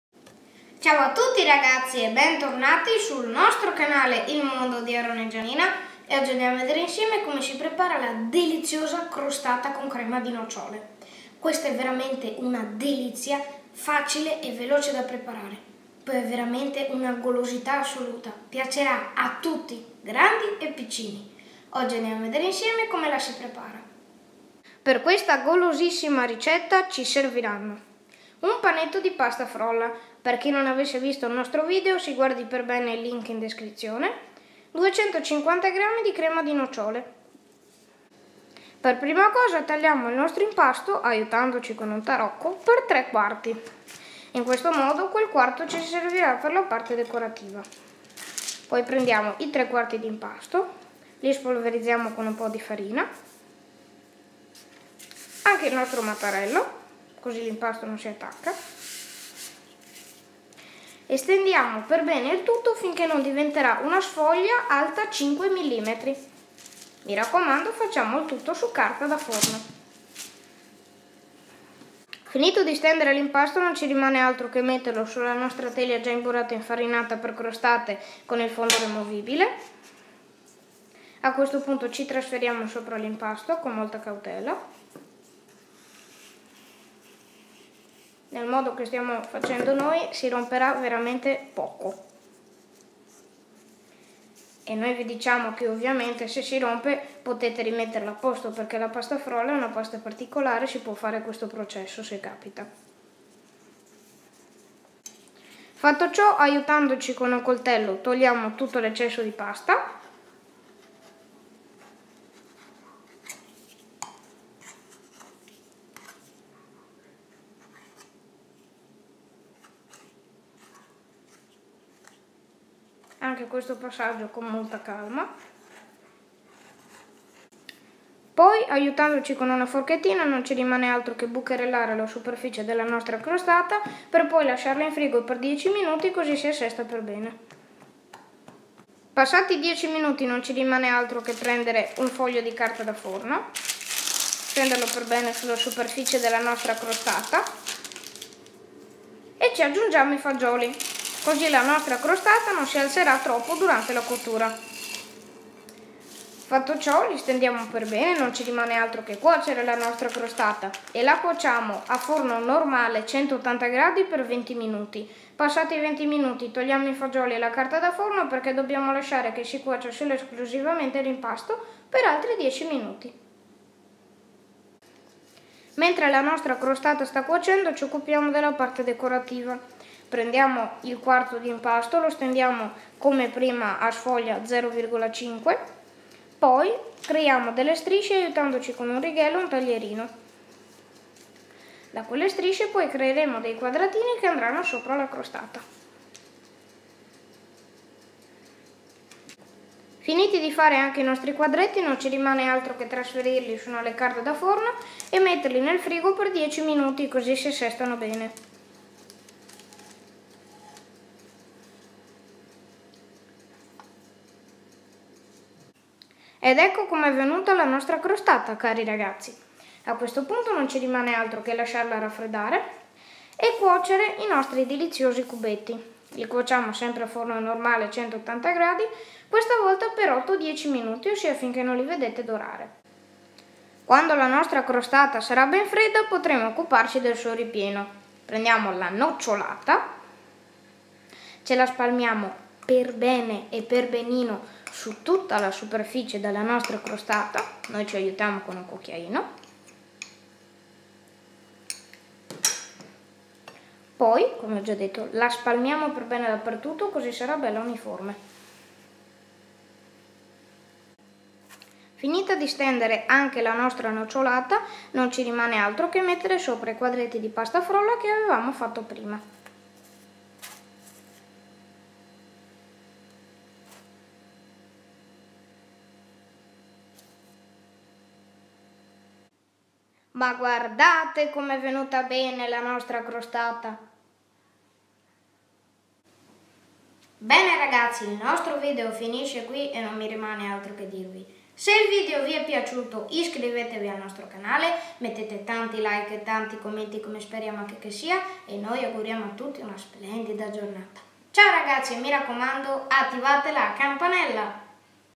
CROSTATA alla CREMA DI NOCCIOLE cremosa e perfetta!!
Ciao a tutti, oggi vediamo assieme come si prepara un dolce molto conosciuto e amato da tutti: la crostata al cioccolato. Questa delizia sarà di sicuro apprezzata da grandi e piccini per la suo incredibile scioglievolezza e il gustoso sapore.

Per questa deliziosa ricetta ci serviranno:
1 panetto di pasta frolla
250g di crema di nocciole

🇺🇸 For this delicious recipe we will need: 🇺🇸
1 shortcrust pastry
250g of hazelnut cream